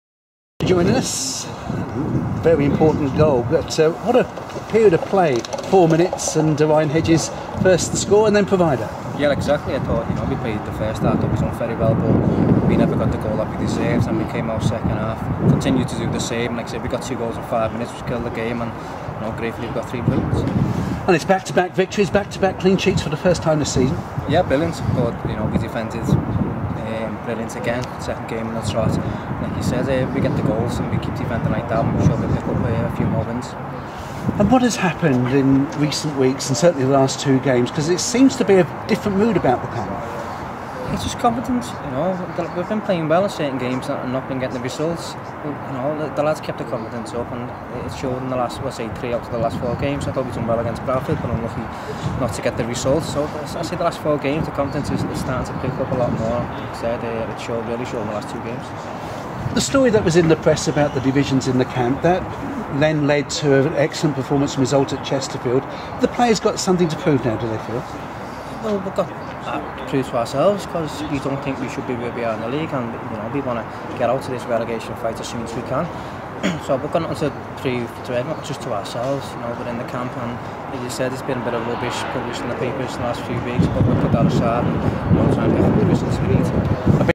Leyton Orient striker Chris Dagnall on 2-0 win at Walsall
Leyton Orient striker Chris Dagnall on 2-0 win at Walsall, and notching his 10tth goal of the season in the process.